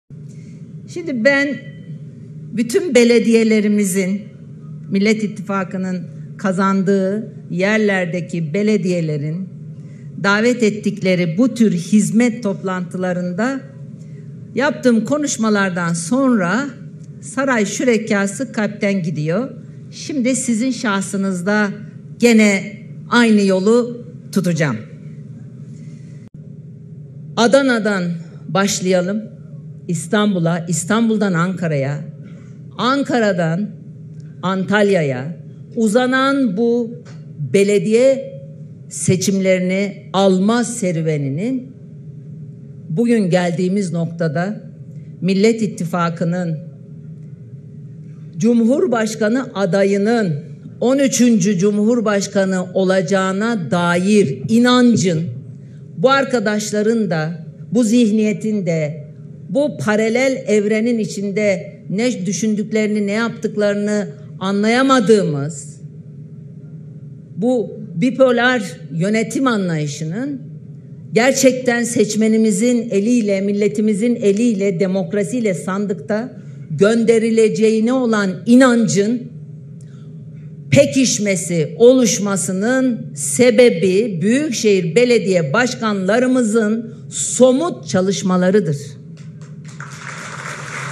Meral Akşener, Kemal Kılıçdaroğlu ve Mansur Yavaş aynı programda! 'Saray şürekası kalpten gidiyor'
İYİ Parti Genel Başkanı Meral Akşener, CHP Genel Başkanı Kemal Kılıçdaroğlu, Ankara Büyükşehir Belediye Başkanı MAnsur Yavaş Ankara Büyükşehir Belediyesi'nin Yeni Otobüslerin Teslimat Lansmanı programına katıldı.